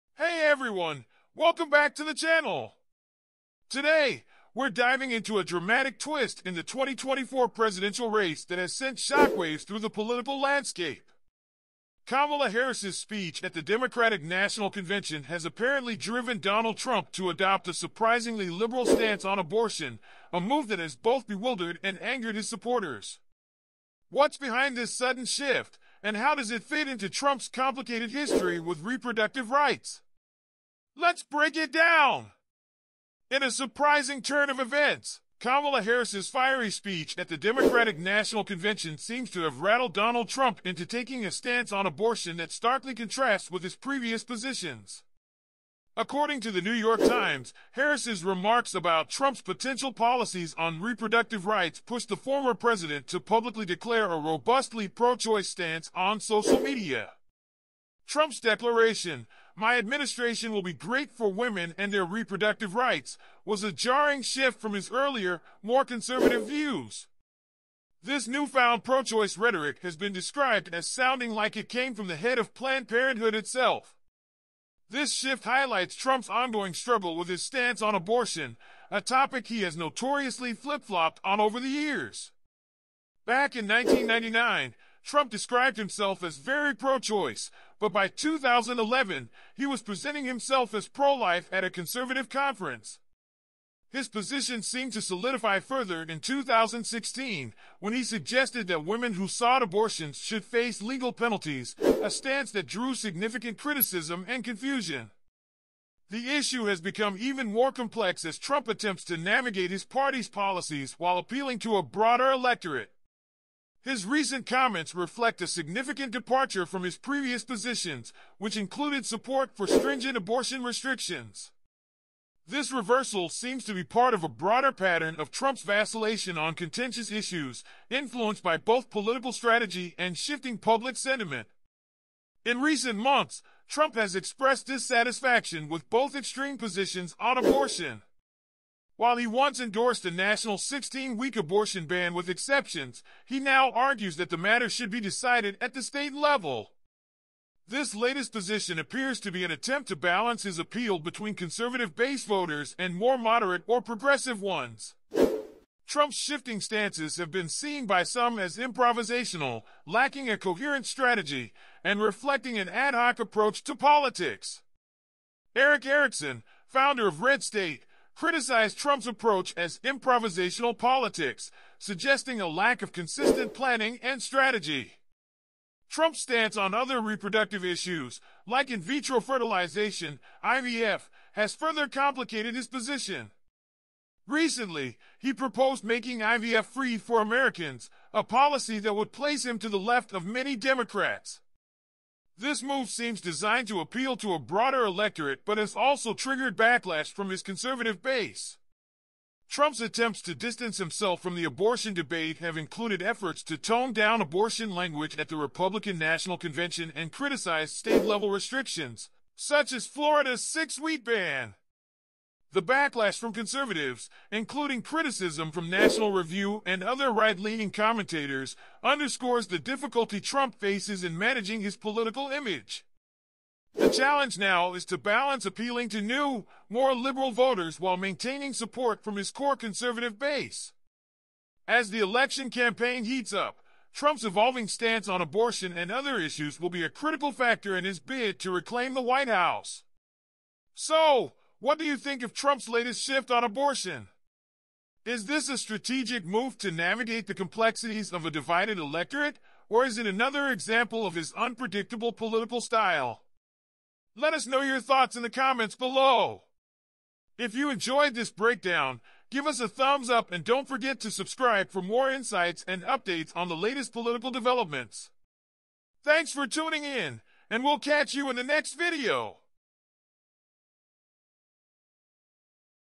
Kamala Harris' Tactic Pushes Trump to Make Shock Statement on Fluctuating Election Issue
Hey everyone, welcome back to the channel! Today, we’re diving into a dramatic twist in the 2024 presidential race that has sent shockwaves through the political landscape. Kamala Harris’s speech at the Democratic National Convention has apparently driven Donald Trump to adopt a surprisingly liberal stance on abortion – a move that has both bewildered and angered his supporters. What’s behind this sudden shift, and how does it fit into Trump's complicated history with reproductive rights? Let’s break it down.